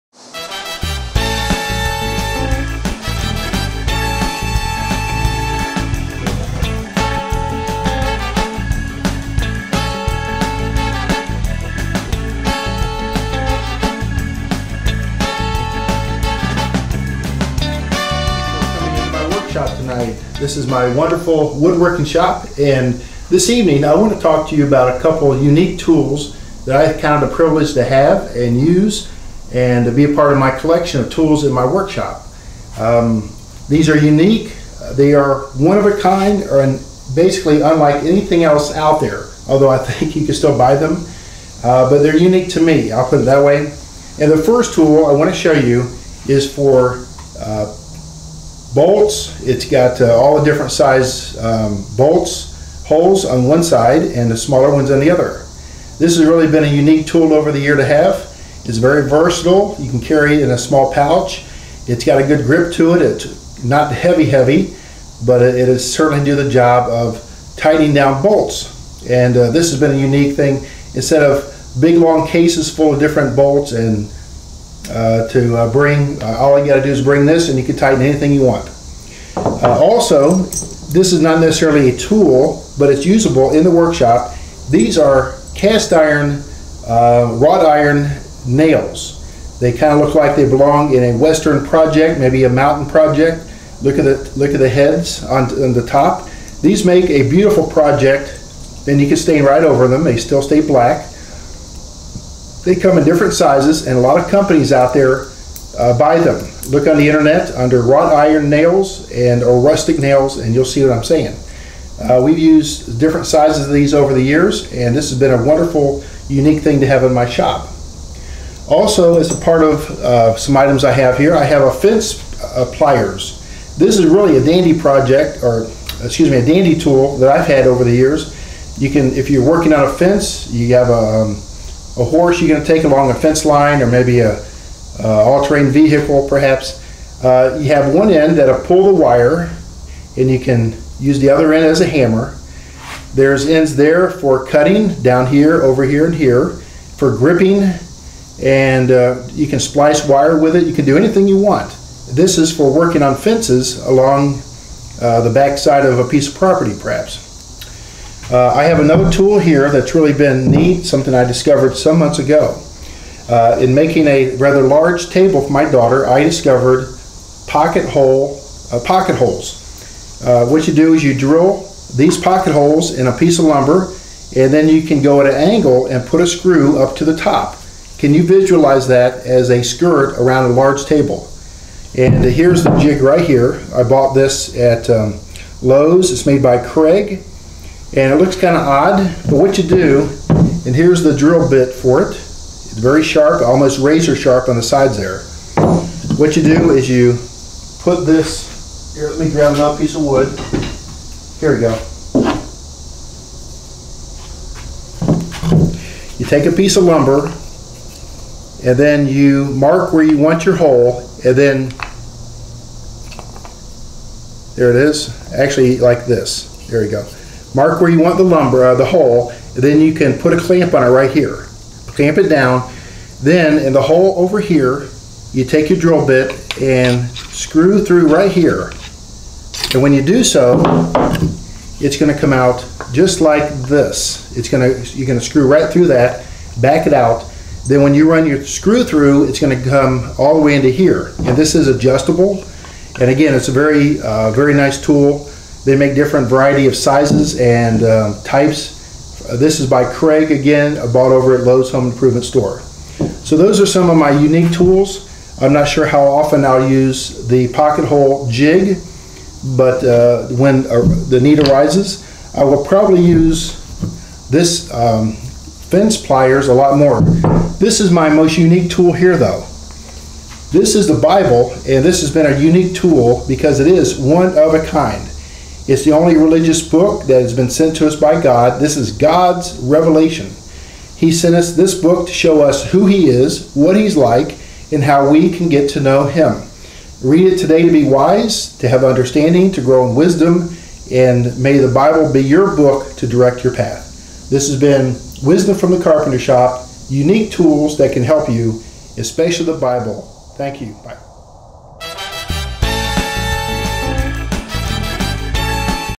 UniqueTools: Wisdom from the Carpenter's Shop
In my wood shop I have many unique tools. By unique I mean different but one tool is a "one of a kind" type tool. Which one is it? Watch and find out.